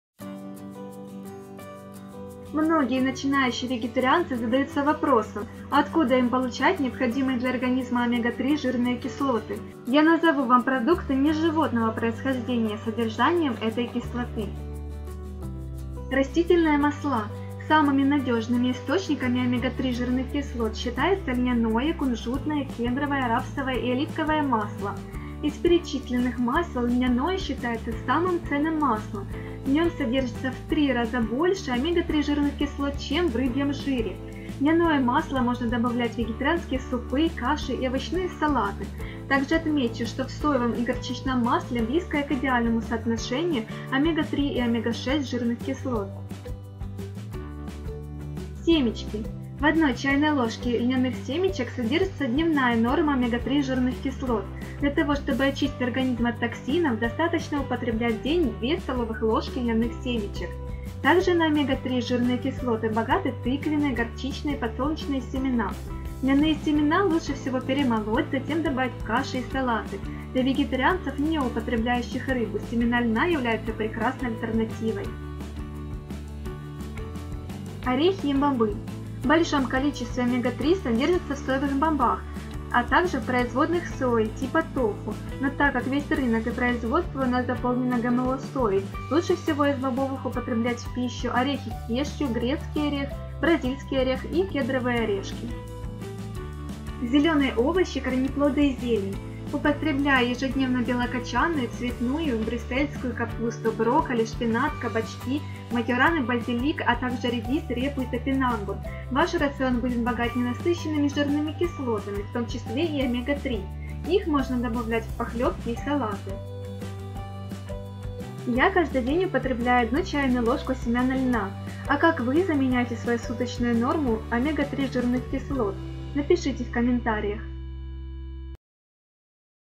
Где брать Омега 3 вегетарианцам\ веганам\ сыроедам? Продукты растительного происхождения
Омега 3 жирная кислота  - это очень нашумевшая тема. Нам говорят, что она содержится только в животных продуктах,рыбе.
Но так ли это?
Правда ли, что семена льна могу заменить ее суточную норму?
самое интересное тут